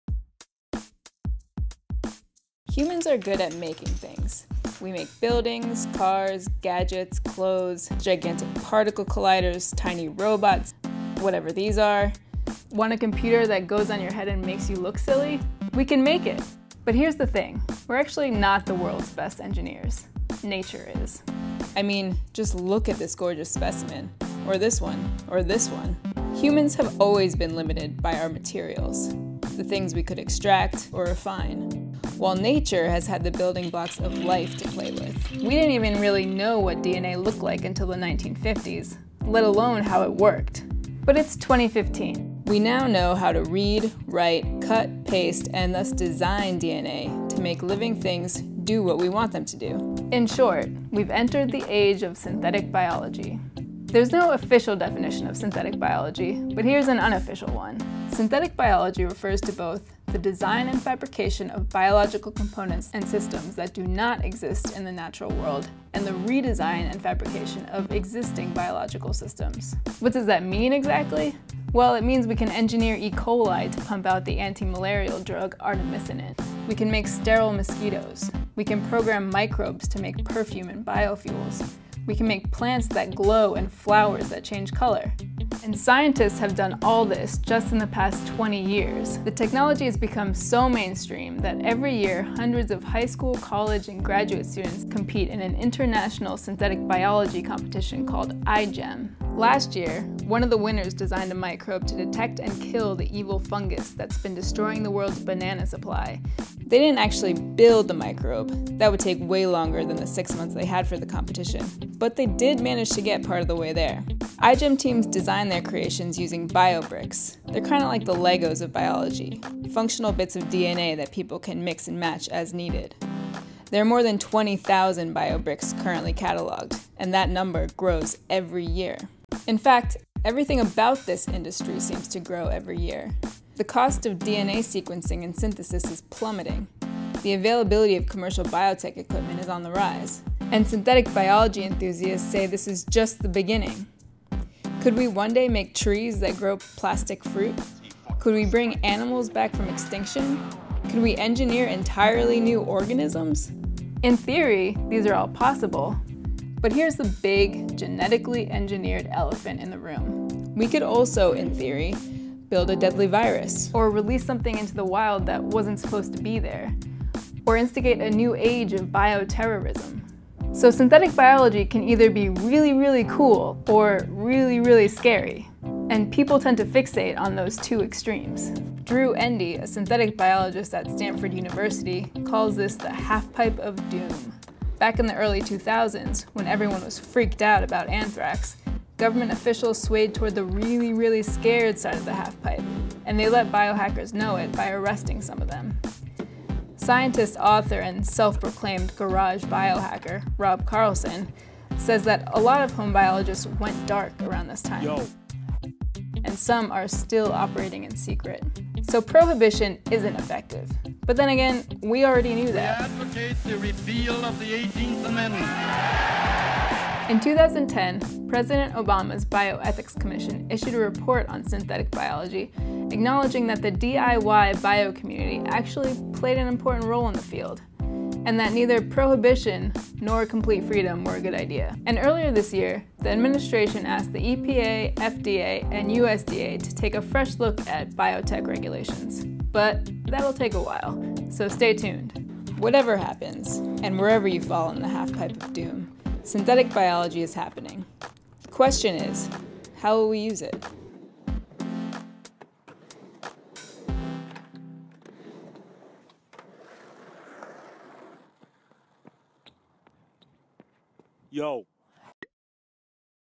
Synthetic biology, explained
That’s because there’s always been this divide between technology — the inanimate stuff that we build — and biology — the living stuff that nature builds. It’s not that we can’t build living things; we just have to figure out how. And that’s exactly what synthetic biologists are trying to do. Check out the video above to see how.

featuring: Suzanne Jacobs
producer: Daniel Penner
asst. producer: Ana Sofia Knauf
media:
goo.gl/VvA0kV
goo.gl/xihlzh
Archive.org
Prelinger Archives
Shutterstock